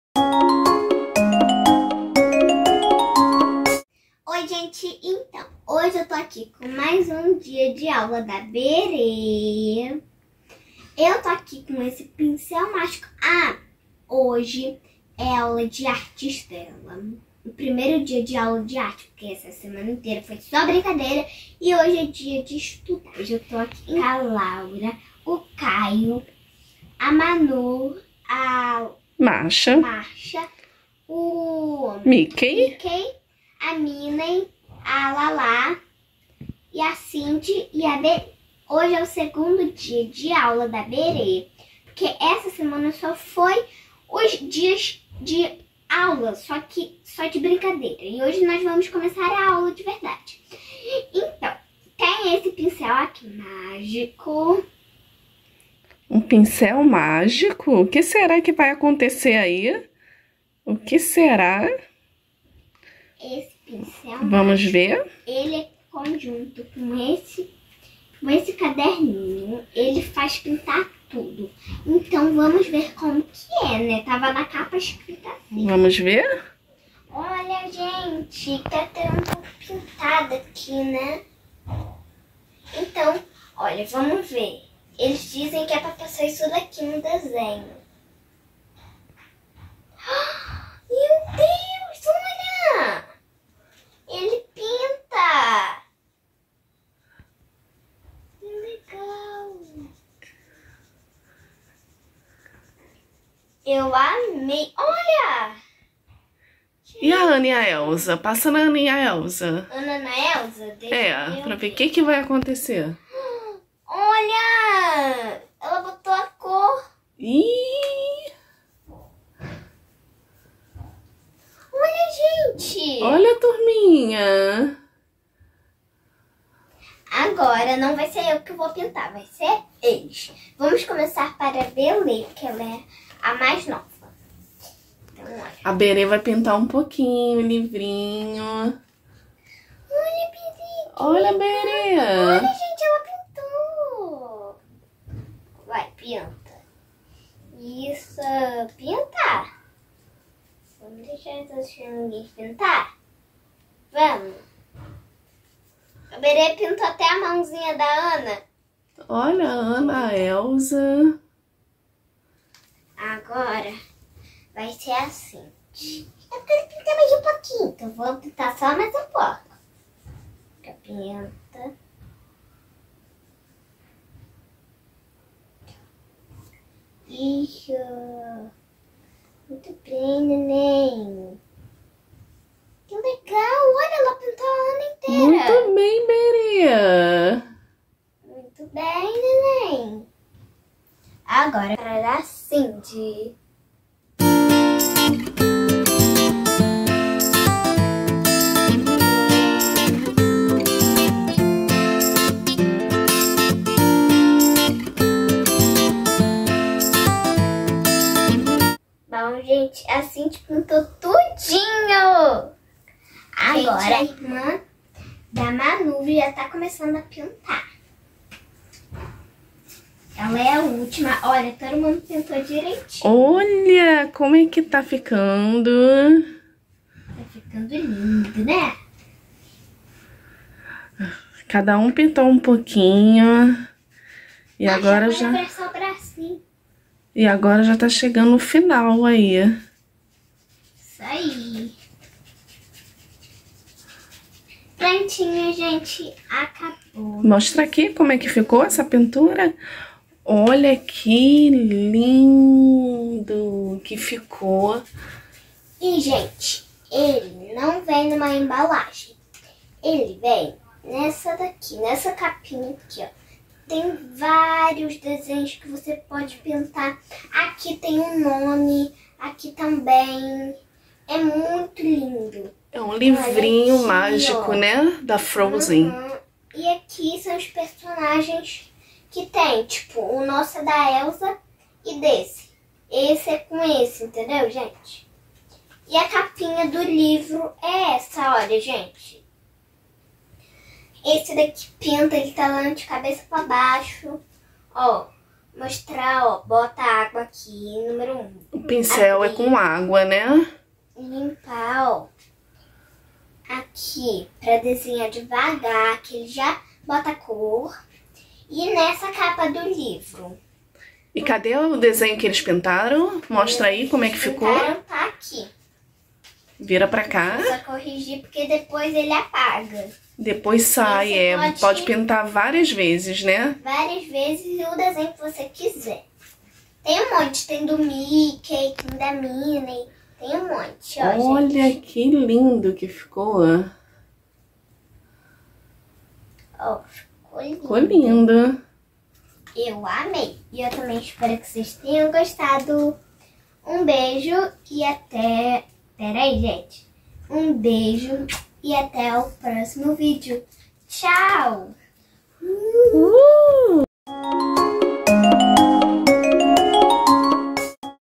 ROTINA DIA DE CRECHE DA BEBÊ REBORN BERÊ / AULA DE ARTES / PINTURA DAS PRINCESAS / FROZEN
Olá galerinha,
Como vocês já viram nos vídeos anteriores, minha bebê reborn foi pra creche e foi muito bem recebida no seu primeiro dia de aula.
No vídeo de hoje vou mostrar mais um pouquinho da rotina na creche.